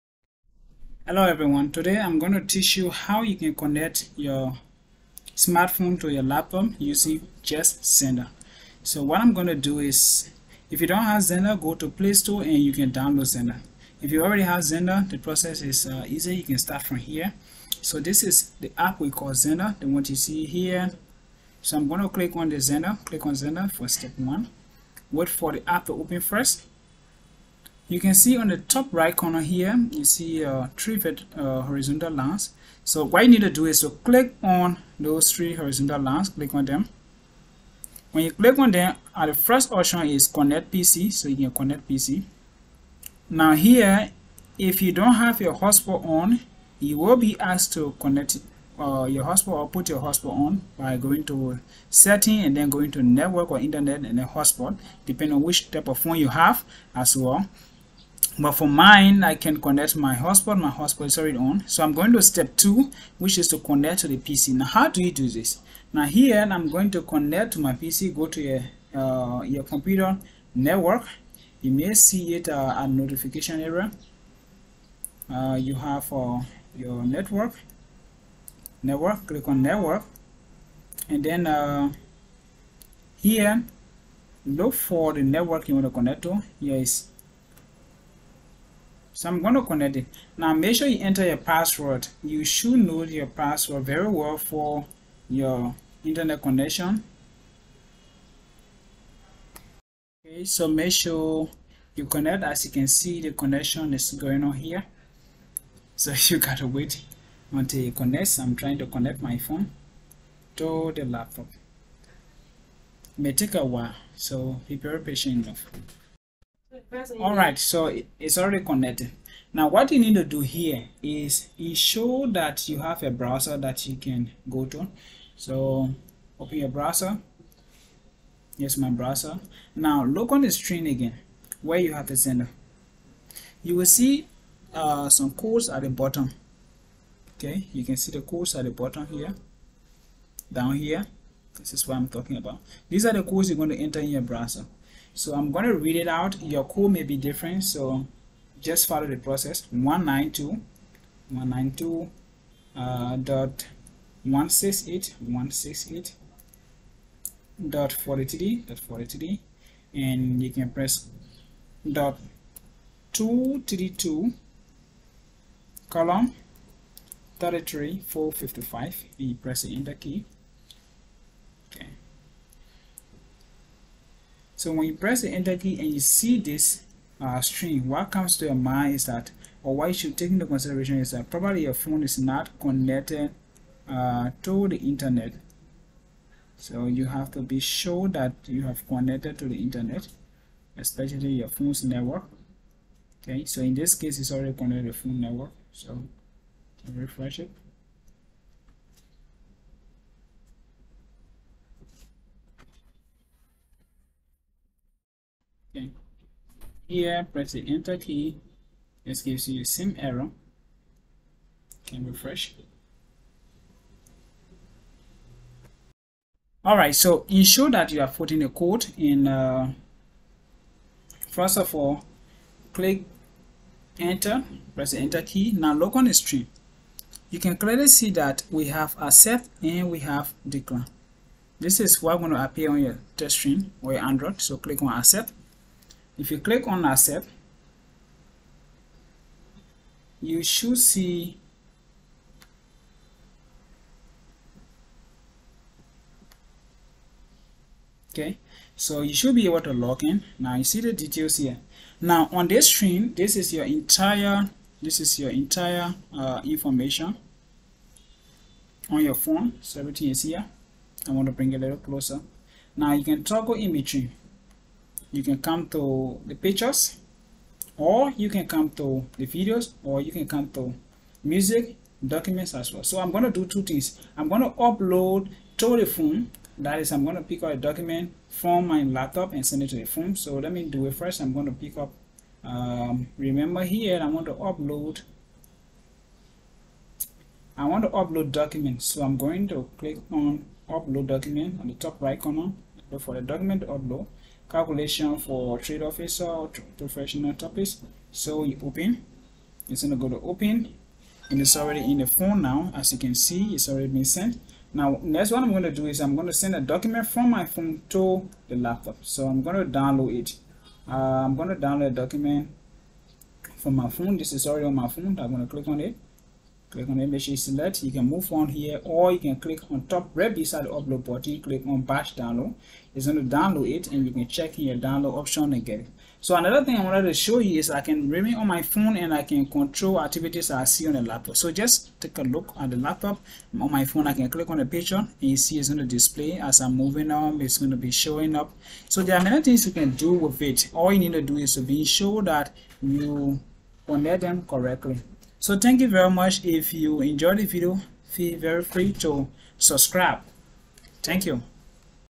WIN 20220510 17 00 54 Pro
Worried about sending huge files from your smart phone to your computer in short time?  If yes, then you are in the right place. This video teaches you how to connect your phone to your laptop while offline.